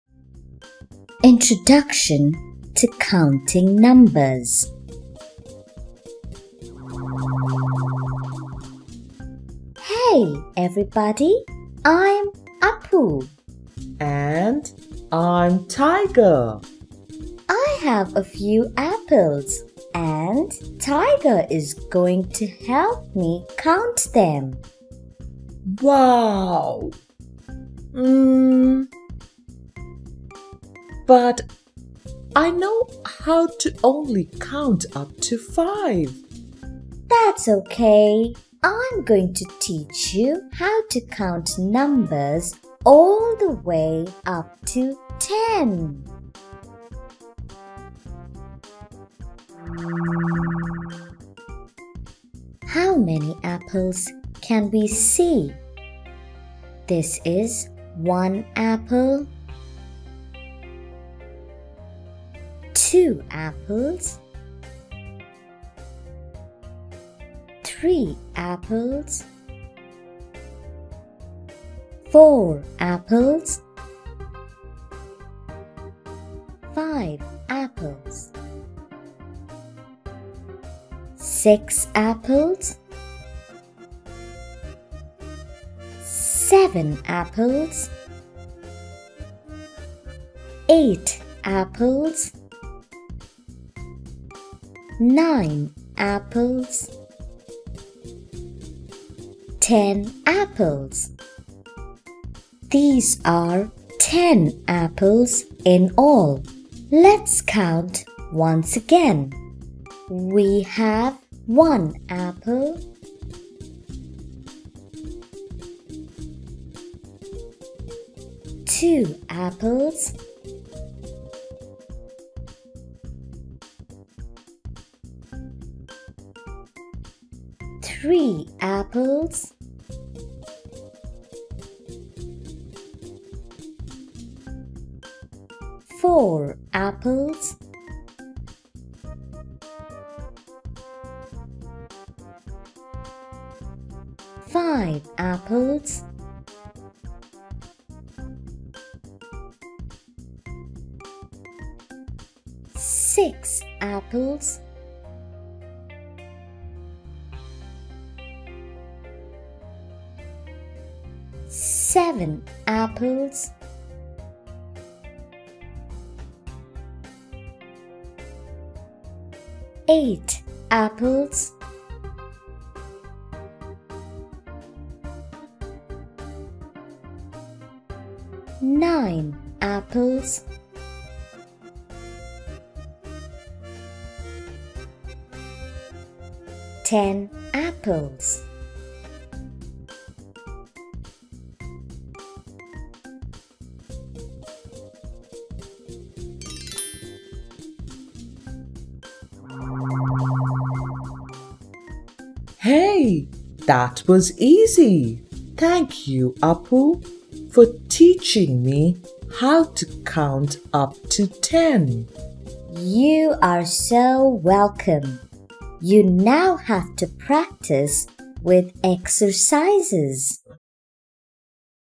K1 - Module 2 - Introduction - Counting Numbers up to 10 | Appu Series | Kindergarten Math Learning
Counting numbers is a breeze with Appu and Tiger by your side!